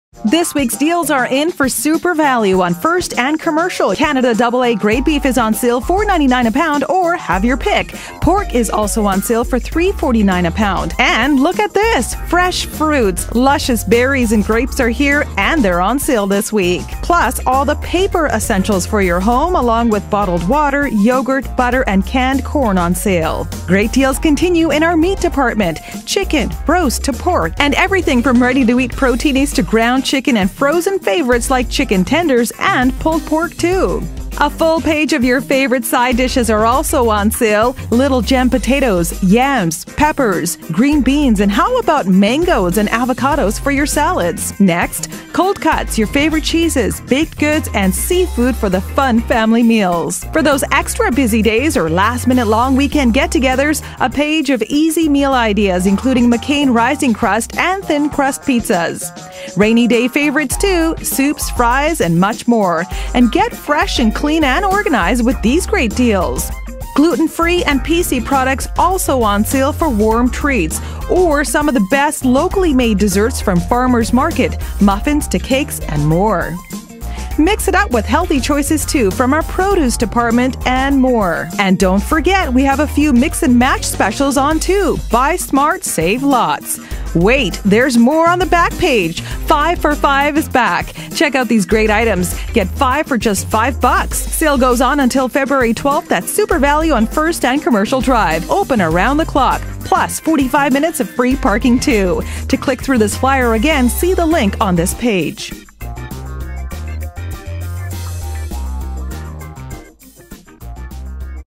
Family Day Long Weekend Flyer Until Feb 12, 2015
Fresh meats, fruits, vegetables, seafood, baked goods, cheeses, dairy products and more on sale this week.  Plus get 5 items for $5 and check out our mix and match deals too.